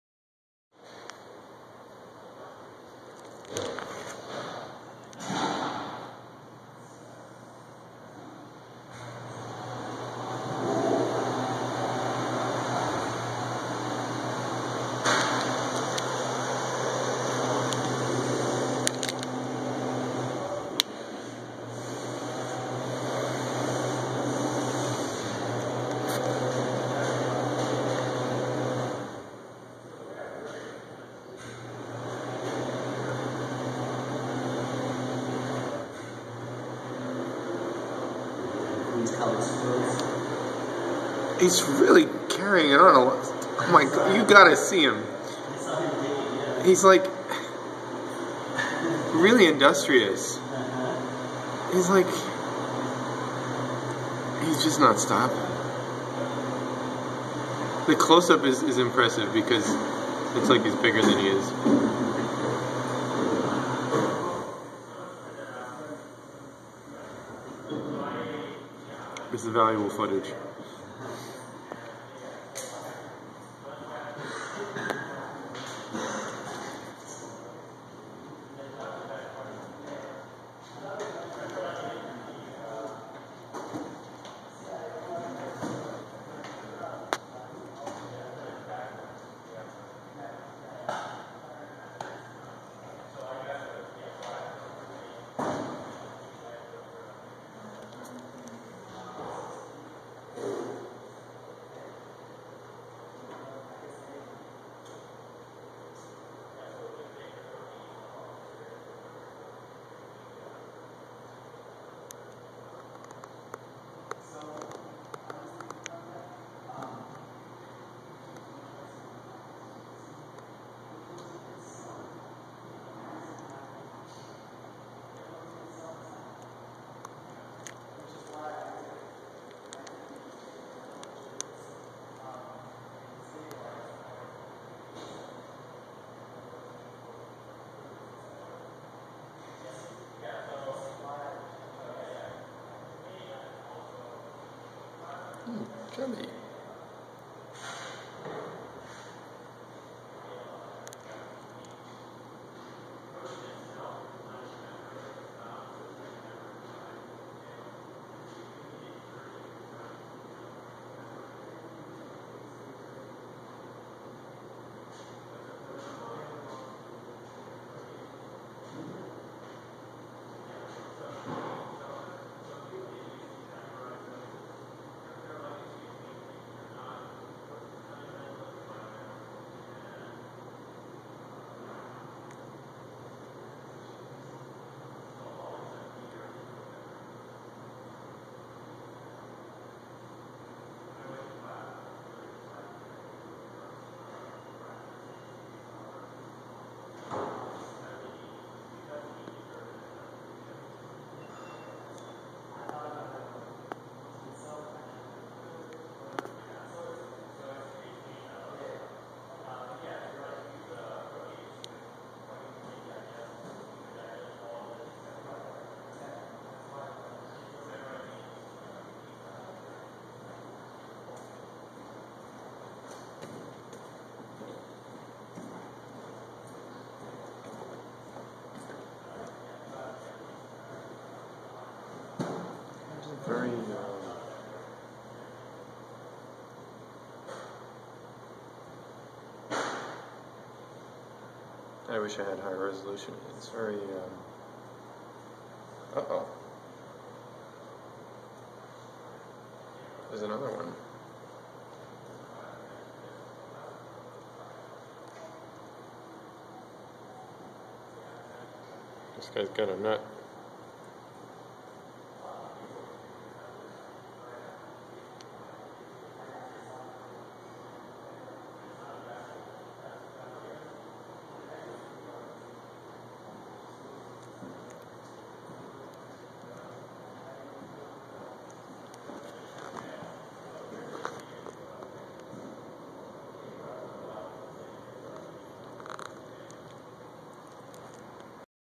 Squirrel! Then another squirrel!!!!
Boring for most people.  You were warned.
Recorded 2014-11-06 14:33
I was fascinated by how industrious this guy was, so I kept filming.  Then there was another squirrel.  He had what looked like an acorn.  I really like these two.  They are going about their business like they aren't aware of each other.  But they must be aware of each other, right?